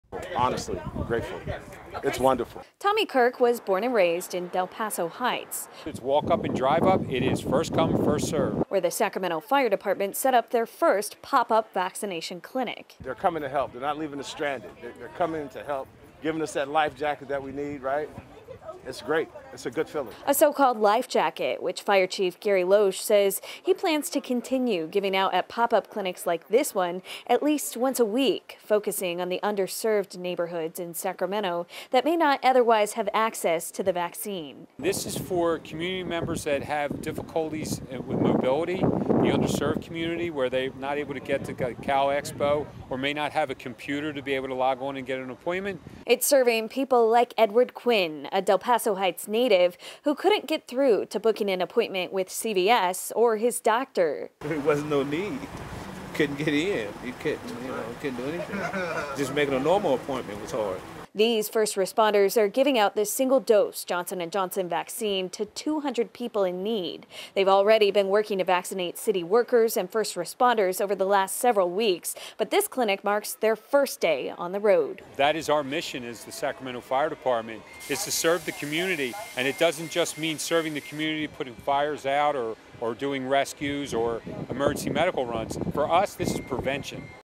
Sacramento Fire holding pop-up vaccine clinic in Del Paso Heights
The Sacramento Fire Department hopes to hold pop-up vaccine clinics in underserved communities at least once a week, using the single-dose Johnson & Johnson vaccine.